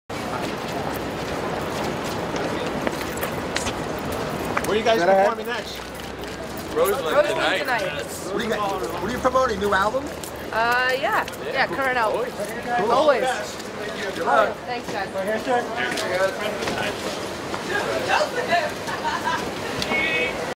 Lzzy Hale at the SiriusXM Radio studio in New York, NY, o...
Lzzy Hale at the SiriusXM Radio studio in New York, NY, on 4/30/13.